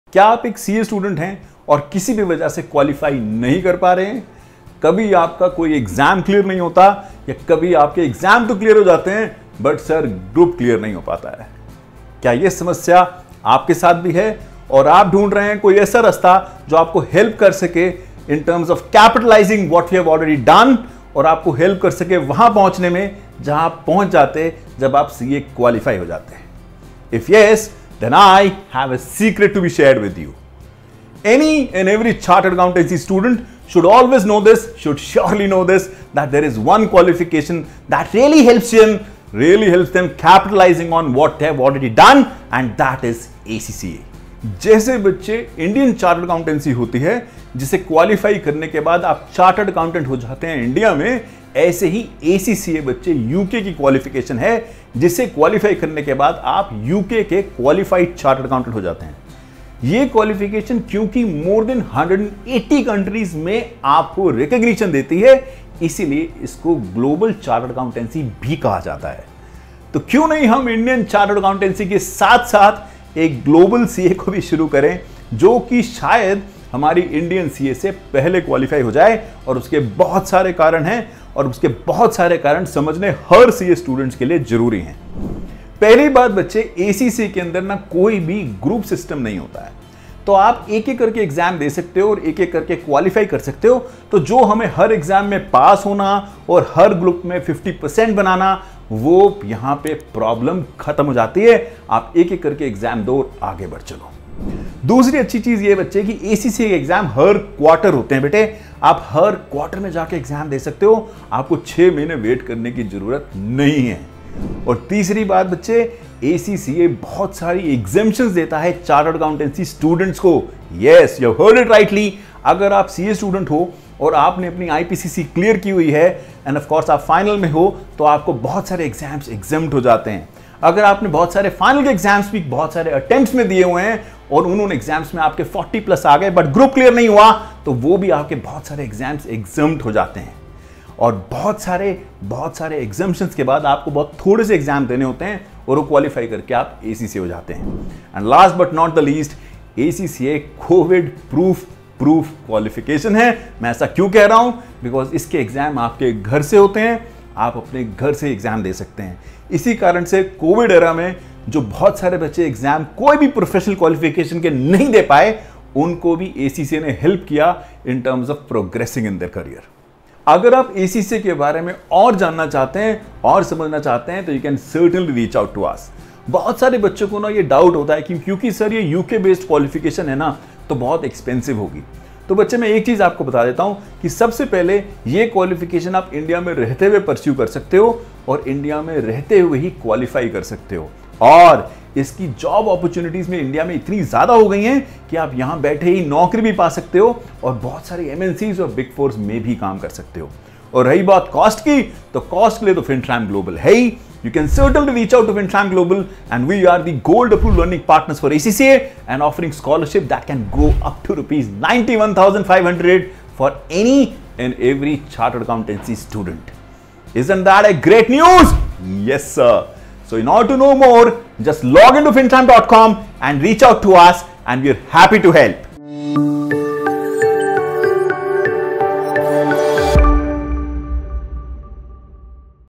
One thing must to know for CA Students | Plan B for CA Students | Career option for CA Students
Hi Guys! In this video, We have discussed about Back up plan for CA aspirants.

So here are some of the good career options which you can pursue , if you are thinking for any plan B.

Hope this video is useful for you.

Join our Telegram Channel to get recent updates on ACCA Exams and recent Job Opportunities